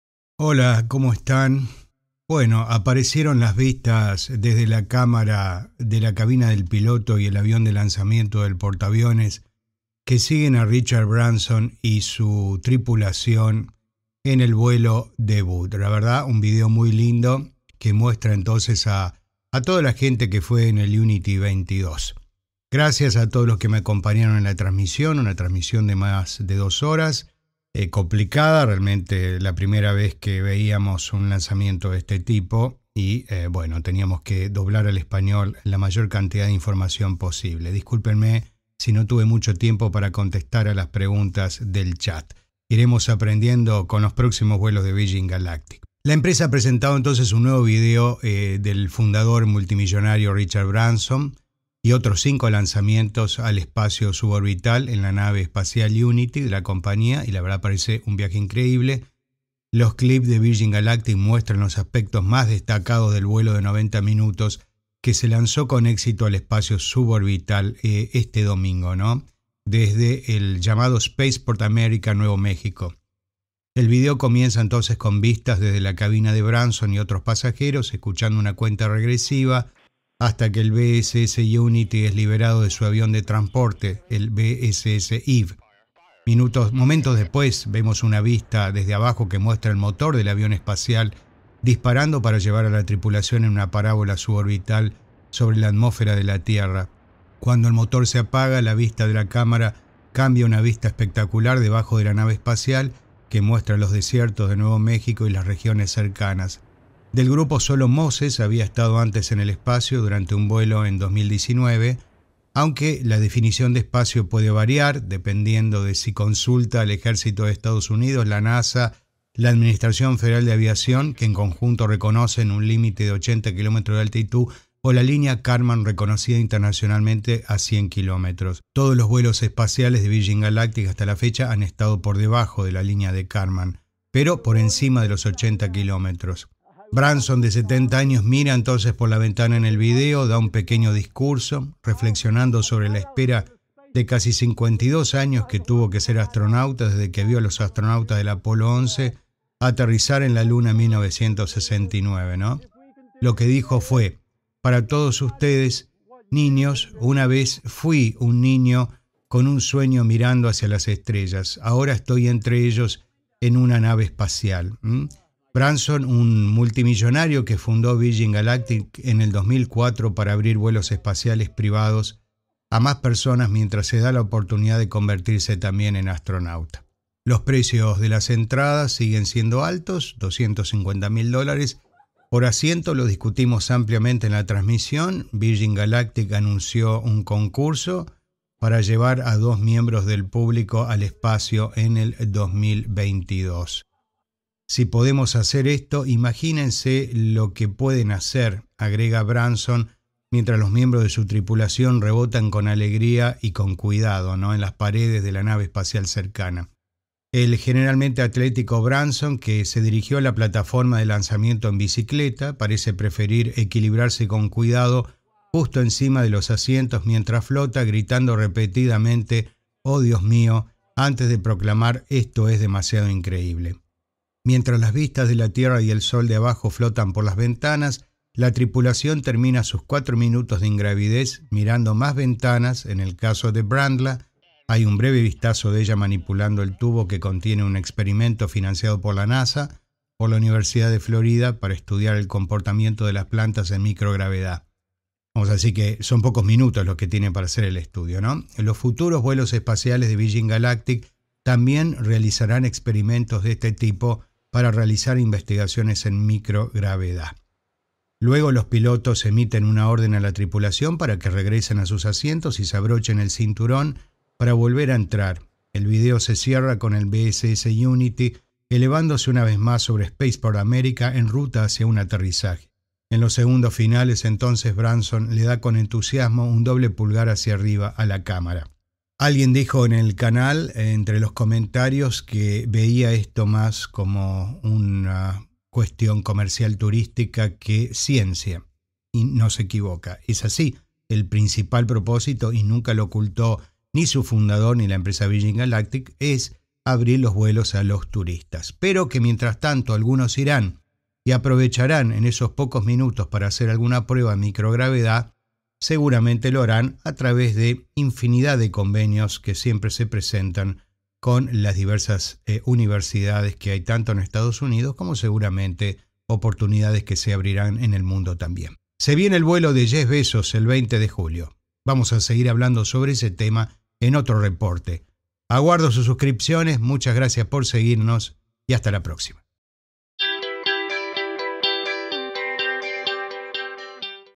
INCREÍBLE VIDEO VIRGIN GALACTIC MUESTRA A LA TRIPULACIÓN DE RICHARD BRANSON EN EL ESPACIO
Nuevas vistas de la cámara desde la cabina del piloto y el avión de lanzamiento del portaaviones siguen a Richard Branson en su vuelo debut.